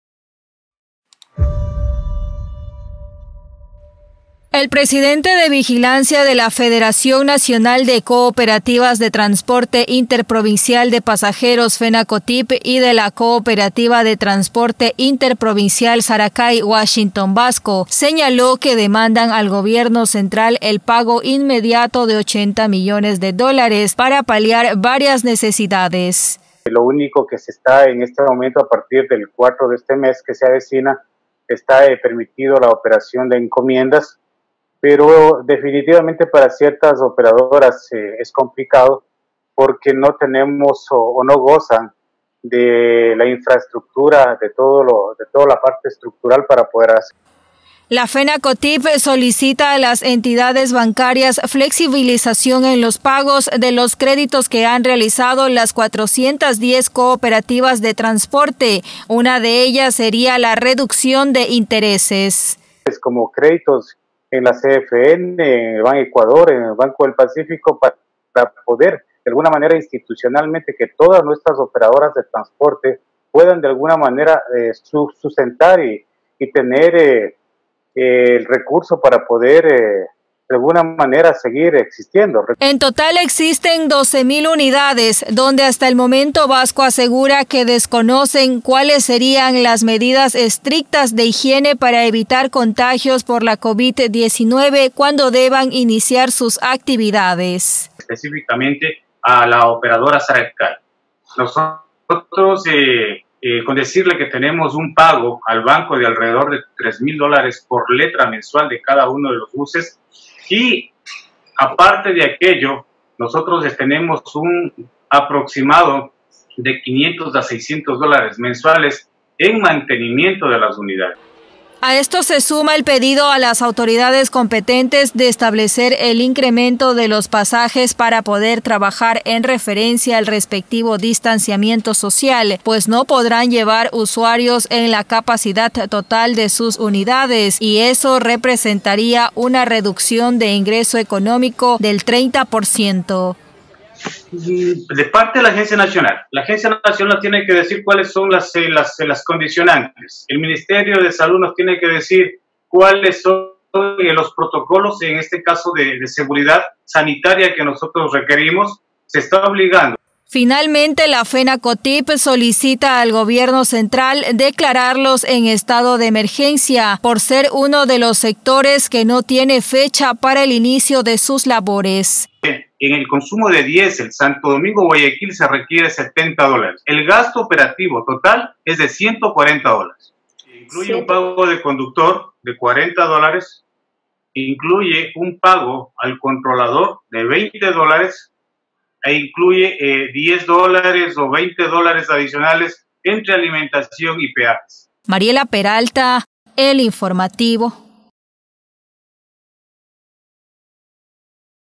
Transportistas solicitan incremento en costo de los pasajes
🔴NOTICIAS: La Federación Nacional de Cooperativas de Transporte Interprovincial de Pasajeros (FENACOTIP), solicita al gobierno central declararlos en estado de emergencia, por ser uno de los sectores que no tiene fecha para el inicio de sus labores.